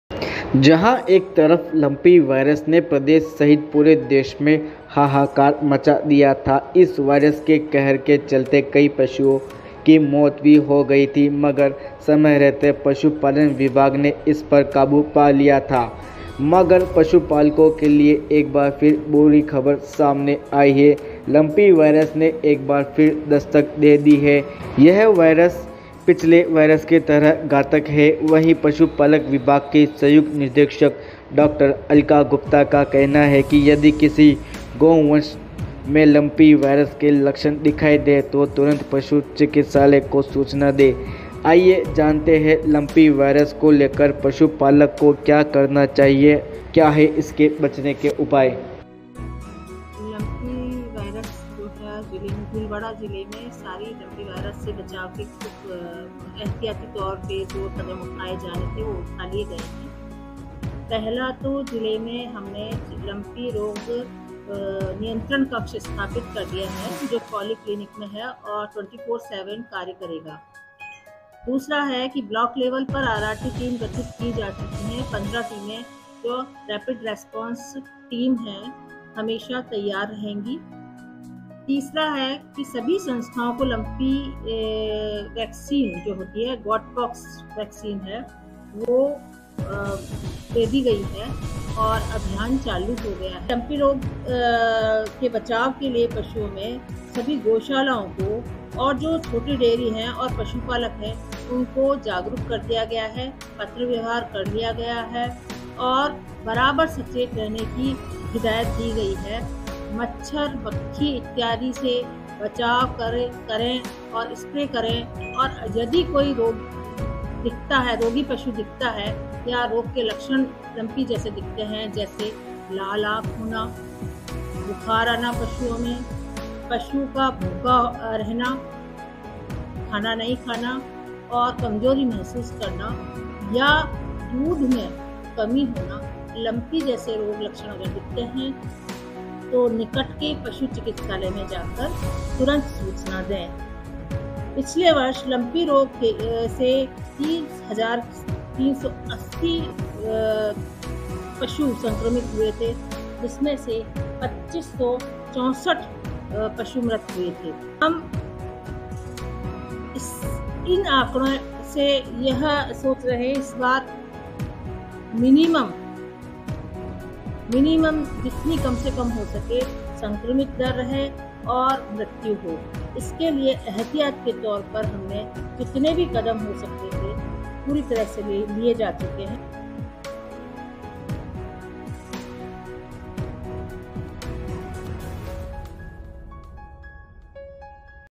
भीलवाड़ा में लंपी वायरस ने फिर दी दस्तक, वेक्सिनेशन का कार्य हुआ शुरू, ऐसे करें पशुओं का बचाव
लंपी वायरस ने एक बार फिर दस्तक दे दी हैं. यह वायरस पिछले वायरस की तरह ही घातक है. वही पशुपालन विभाग के संयुक्त निर्देशक डॉ अलका गुप्ता का कहना है कि यदि किसी गोवंश में लंपी वायरस के लक्षण दिखाई दे तो तुरंत पशु चिकित्सालय में सूचना दें.